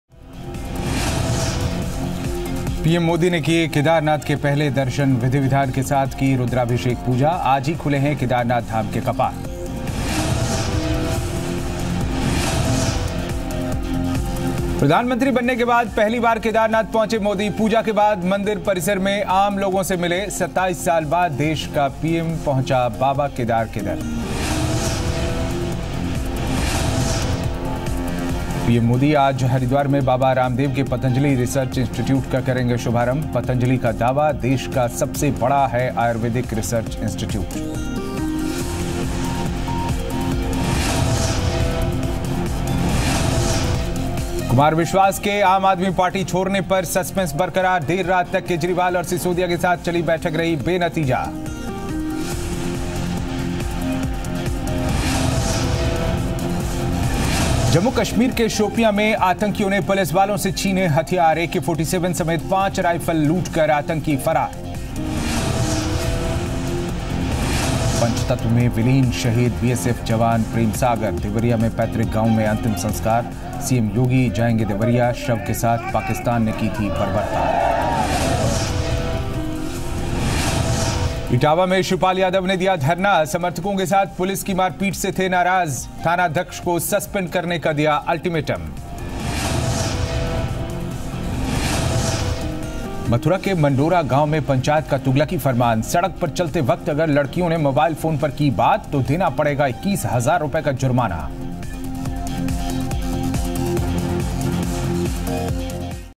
PM Modi offered prayer in Kedarnath temple
PM Modi offered prayer in Kedarnath temple. Watch this special segment and get to know more here.